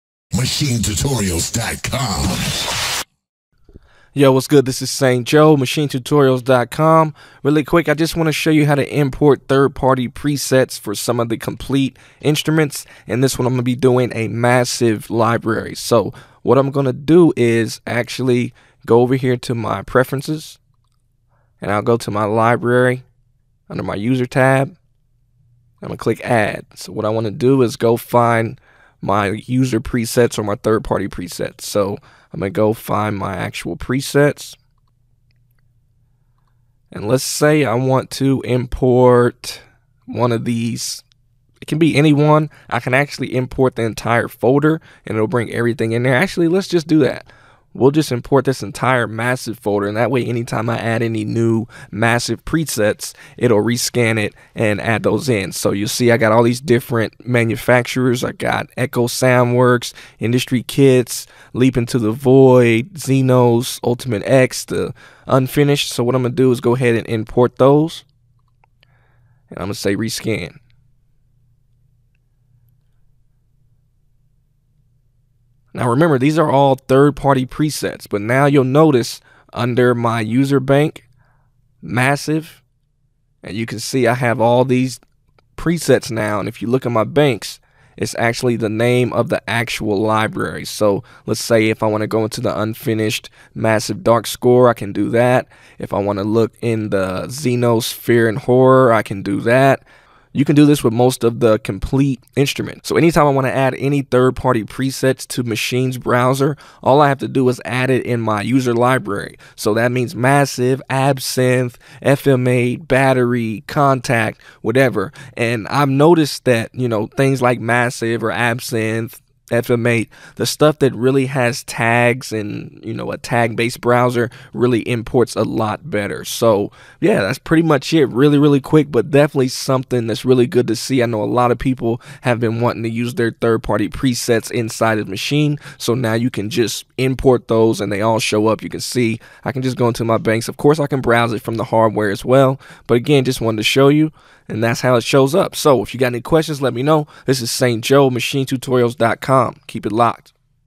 Maschine 2 0 how to import third party MASSIVE presets
The original site for exclusive NI Maschine Training: Quick video showing you how to import all of your third-party or user presets for MASSIVE so they show up directly in the Maschine browser!